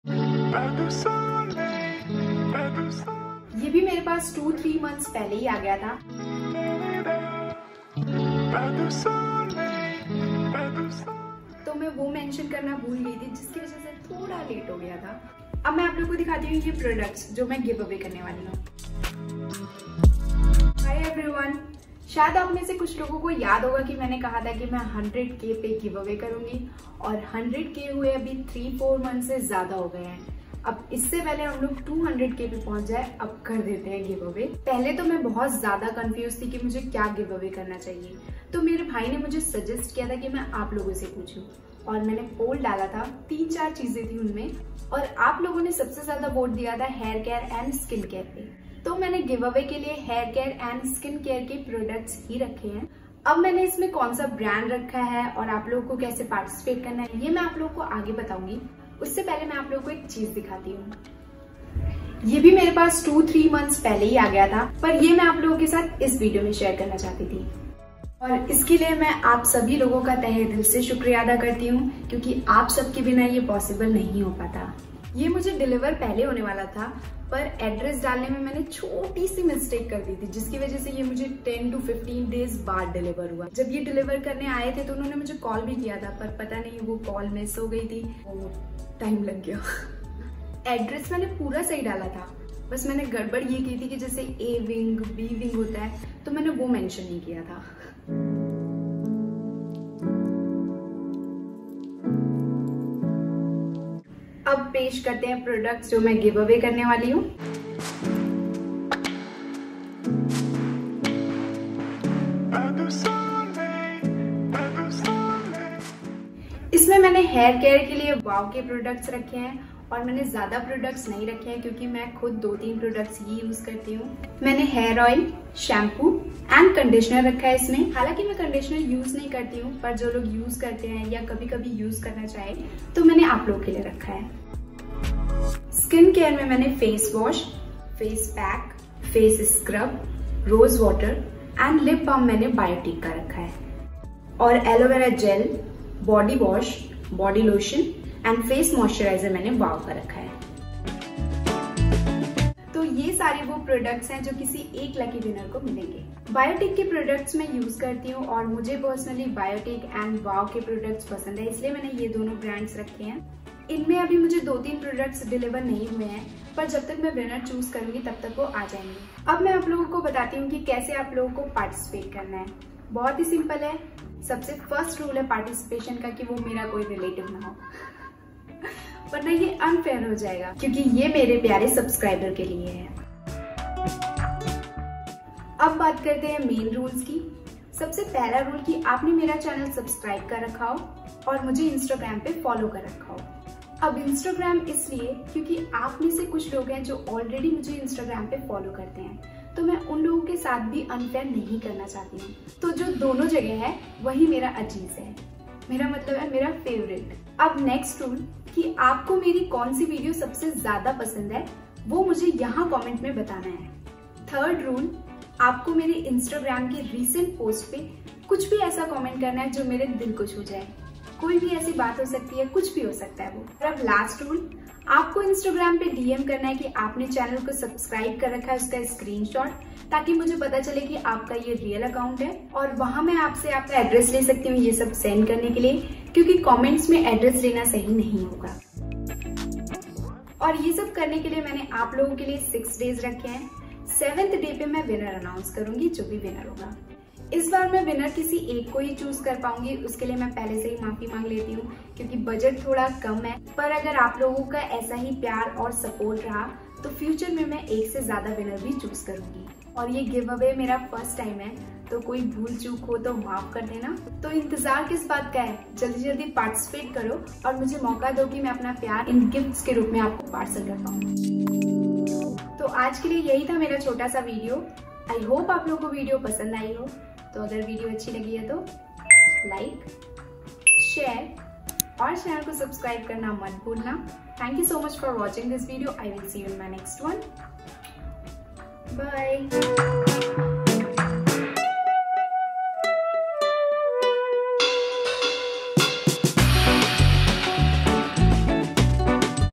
100K Special Giveaway!! & Finally🔥l Got My Silver Play Button | Mishti Pandey
Hello all!!💛

This is very special video! I’m eternally grateful to all of you for making it happen!

3. Comment anything that touches my heart  on my Instagram’s recent post.

4. After doing all this send me the screenshot on Instagram.

And I will update about the winner announcement Shortly!